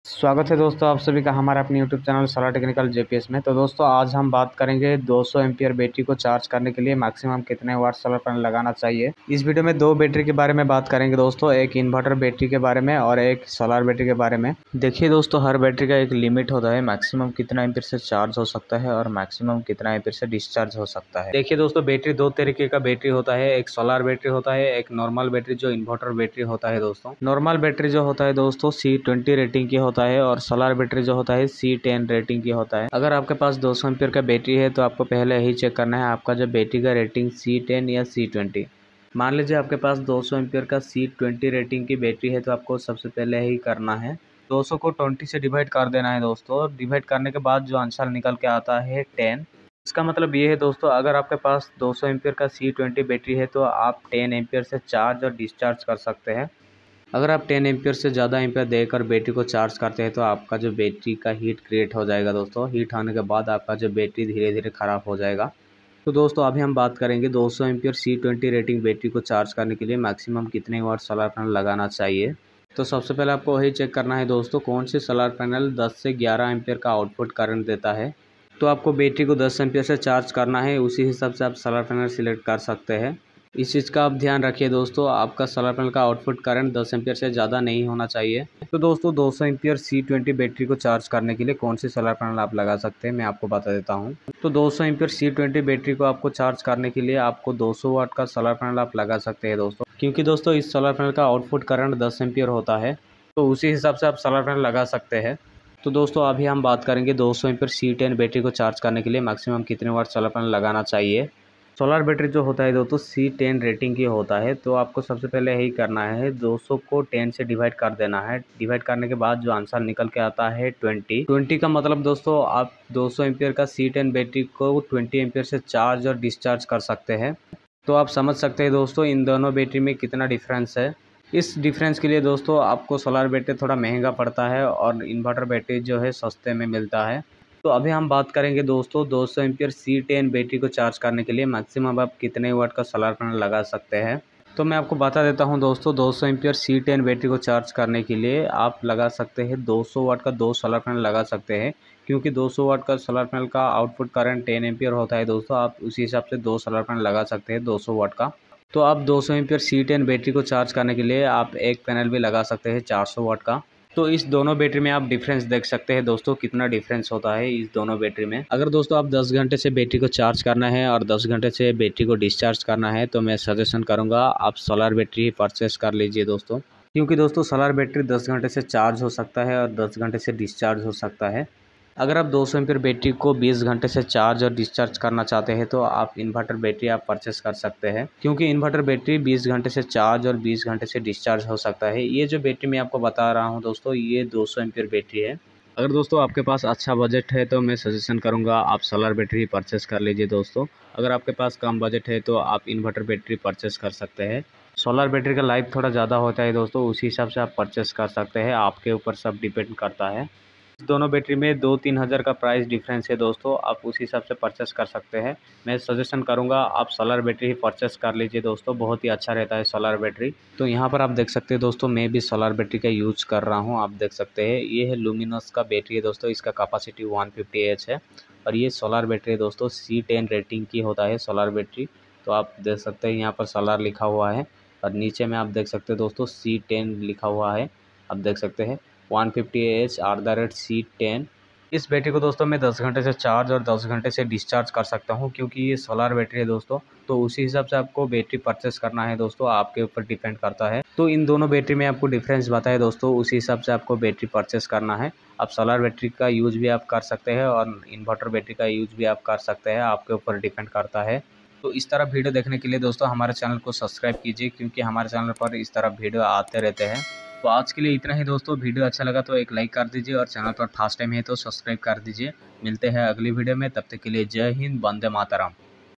Solar Panel For 200Ah Battery
Sathio Main Apko is video main 200Ah Battery Ko Charge karne ke liye kitna Solar Panel Chahie Main Aapko Bataya Hoon App Video ko end tak Jaroor Dekhie